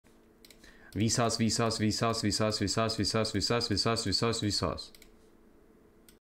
I Said Vsauce 10 Times
Saying Vsauce 10 Times